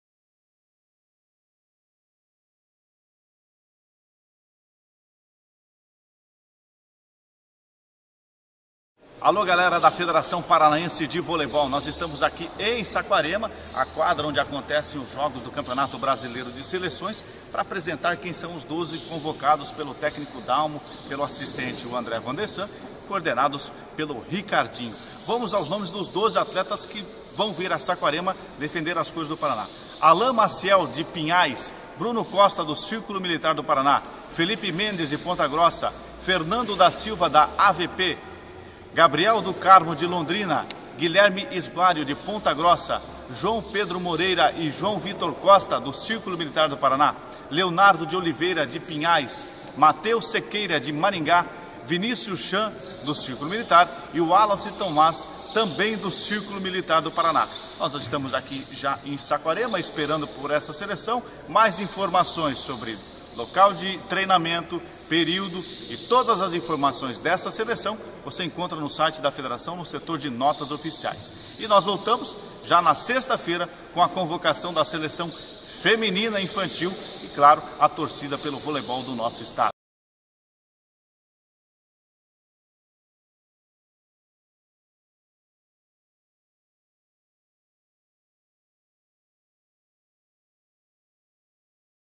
Convocação PR Seleção Infantil Masculina 16 09~1
Convocação da Seleção Paranaense Infantil Masculina para a disputa do Campeonato Brasileiro de Seleções de 13 a 18 de outubro.